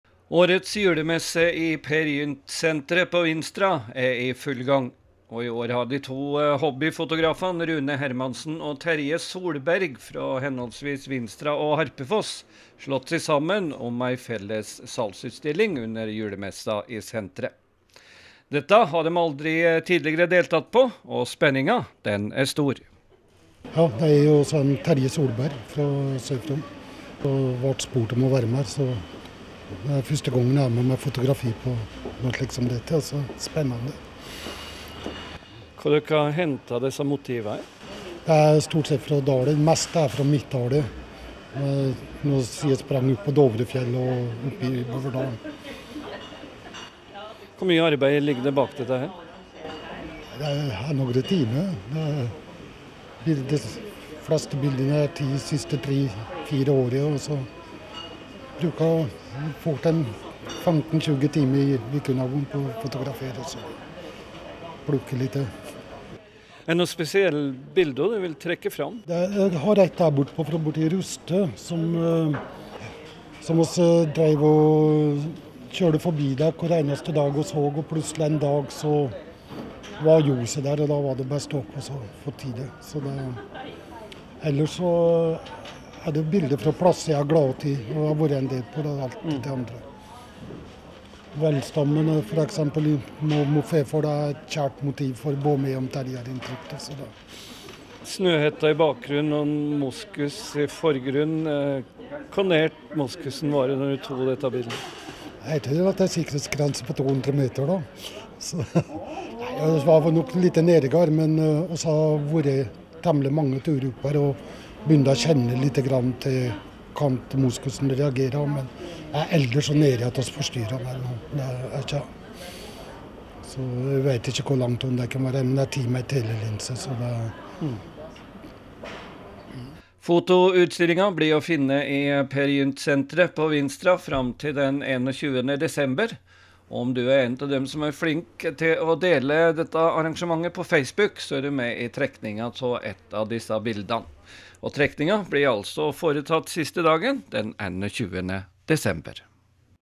Fotoutstilling på Vinstra
De to amatørfotografene Rune Hermansen og Terje Solberg fra Gudbrandsdalen debuterer med egen fotoutstilling under årets julemarked i Peer Gynt Kjøpesenter på Vinstra i desember.